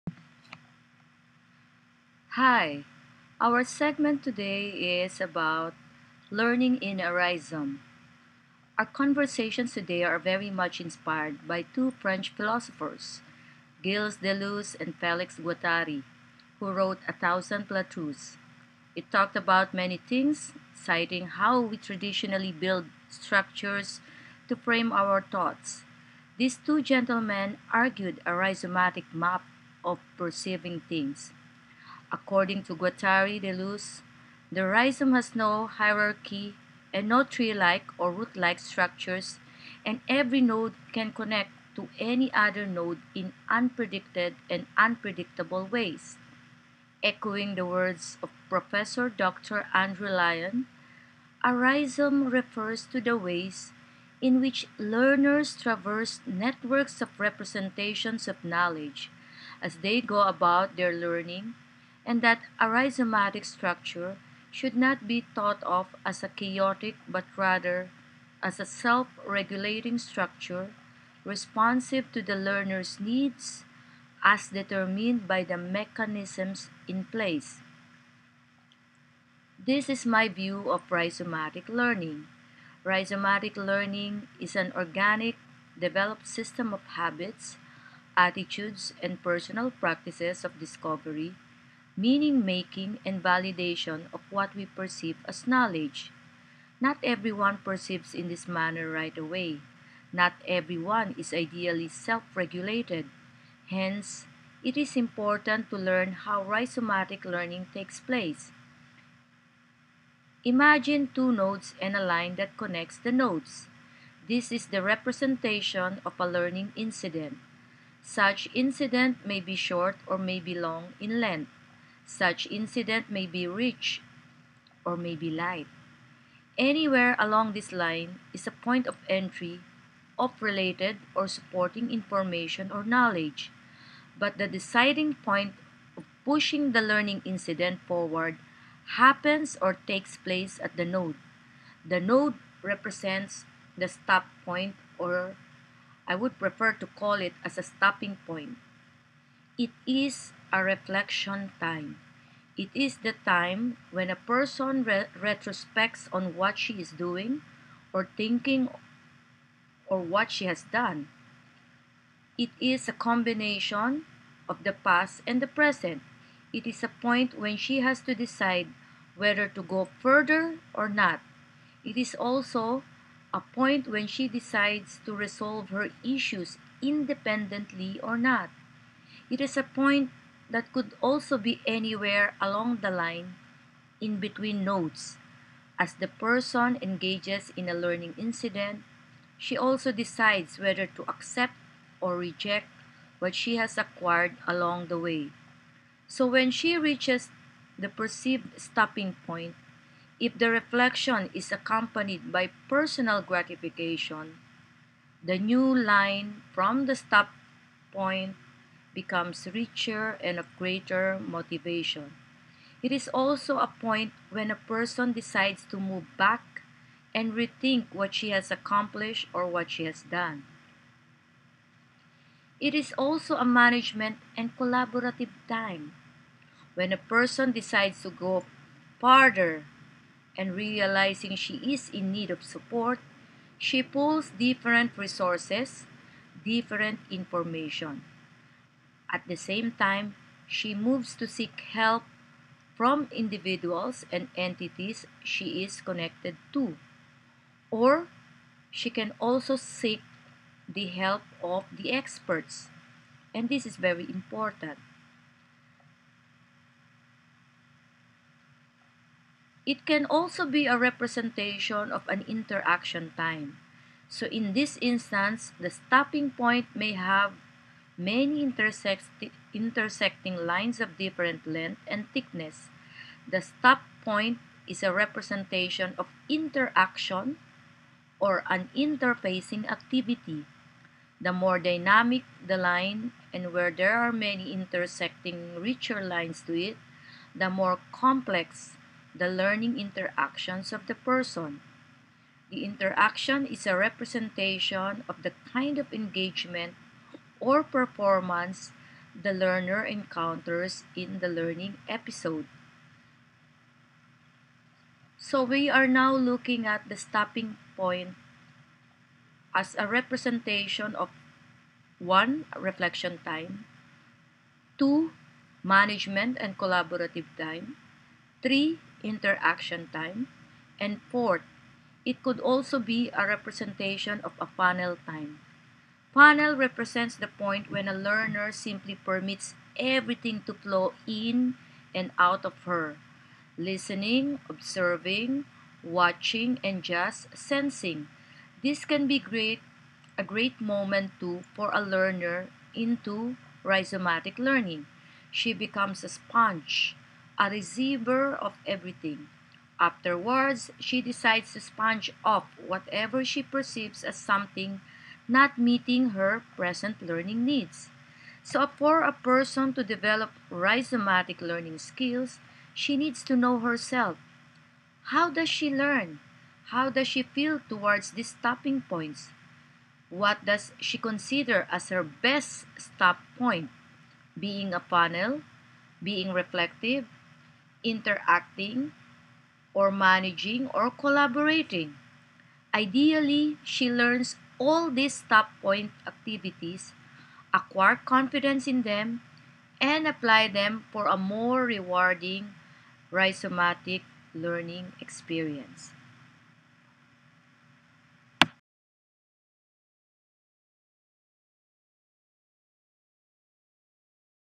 Learning in a Rhizome (compact version)
This discussion dwells on the influence of technology to the many ways individuals pull information and engage in learning in rhizomatic movements.